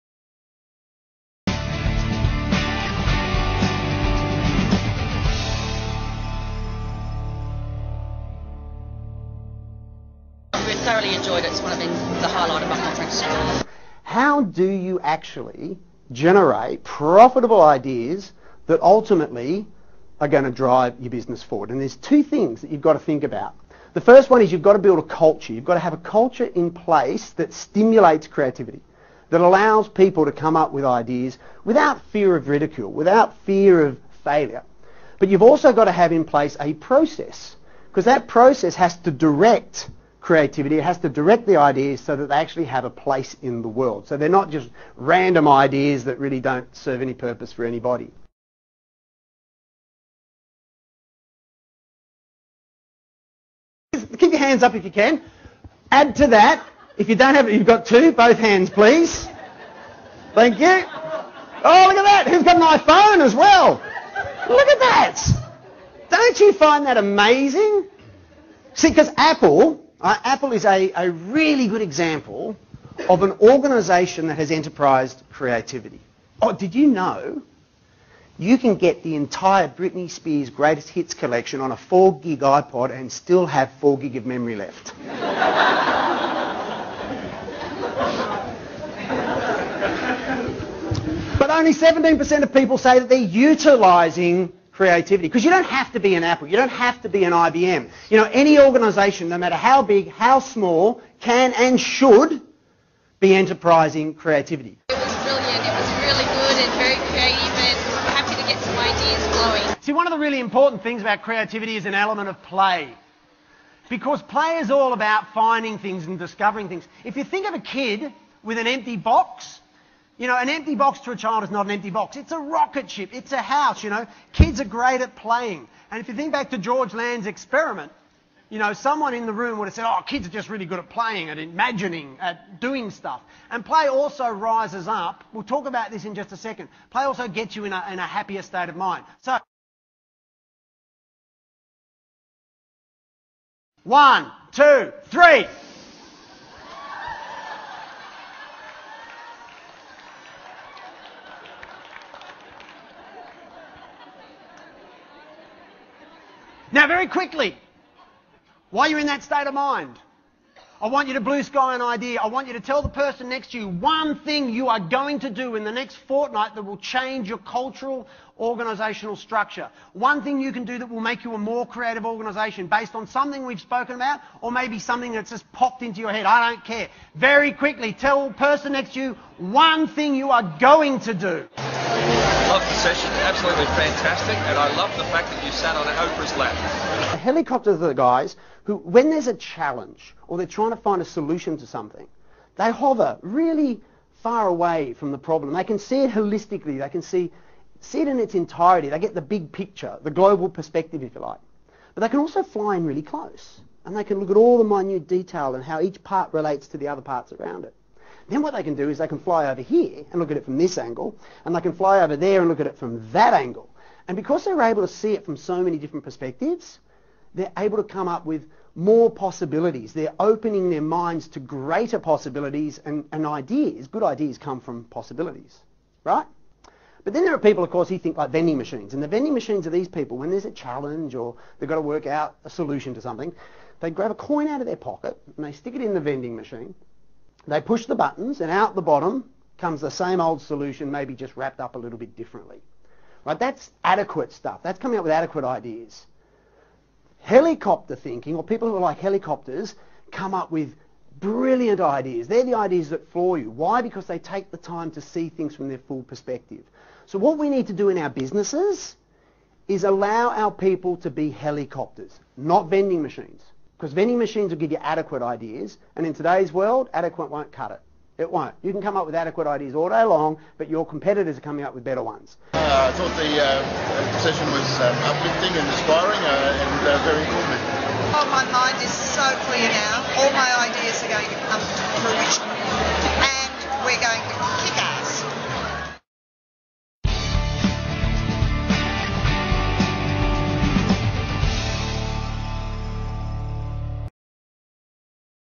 Nigel Collin | Great Expectation Keynote Speaker
Nigel Collin is a highly sought after speaker on business creativity.  He is also an acclaimed author, facilitator and speaker coach. His extensive business and entertainment experience along with his high-energy humour and interactivity make him one of the country's leading presenters.

Nigel talks about creativity to positively affect the bottom line, leaving his audiences not only entertained but skilled on how to apply creativity to their work and lives. It is his real knowledge and experience that sets him apart from others.